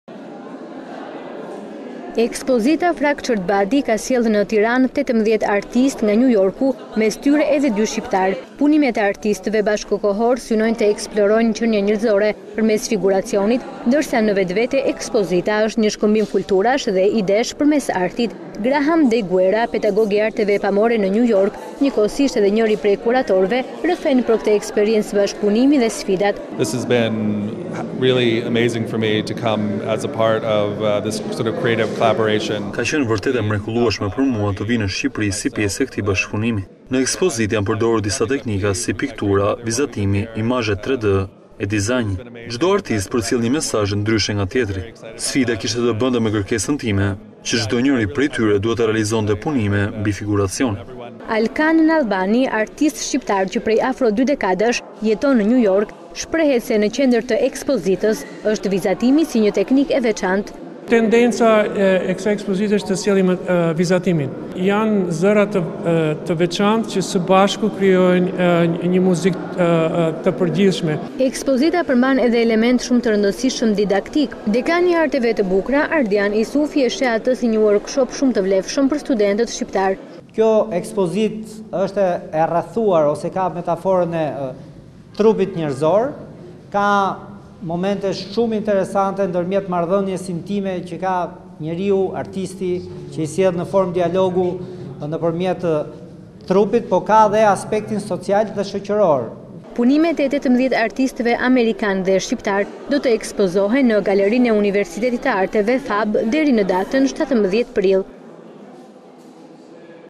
Ora News – Ekspozita ‘Fractured Body’ sjellë në Shqipëri 18 artistë amerikanë
Fractured Body është tema e ekspozitës që ka bërë bashkë  në Tiranë 18 artistë amerikanë dhe dy shqiptarë. Vizatimet e artistëve bashkëkohore kanë në qendër të tyre qënien njerëzore ndërsa sipas kuratorëve ekspoztia është një shkëmbim kulturash e idesh përmes artit.
Ekspozita ‘Fractured Body’ ka sjellë në Tiranë 18 artistë nga New York-u mes tyre edhe 2 shqiptarë. Punimet e artistëve bashkëkohorë synojnë të eksplorojnë qenien njerëzore përmes figuracionit, ndërsa në vetvete ekspoztia është një shkëmbim kulturash e idesh përmes artit. Graham Day Guerra pedagog i arteve pamore në New York, njëkohësisht edhe njëri prej kuratorve rrefen për këtë ekspericen bashkëpunimi dhe sfidat.
‘Ka qënë vërtet e mrekullueshme për mua të vijë në Shqipëri si pjese e këtij bashkëpunimi.Në ekspozitë janë përdorur disa teknika si piktura, vizatimi, imazhet 3d, dizajni. Çdo artist përcjell një mesazh ndryshe nga tjetri, sfida kishte të bente me kërkesën time që çdo njeri prej tyre duhet të realzionte punime mbi figuracionin’
Alkan Nallbani, artist shqiptar që prej afro dy dekadash jeton në New York shprehet se  në qendër të ekspozitës është vizatimi si një teknikë e veçantë.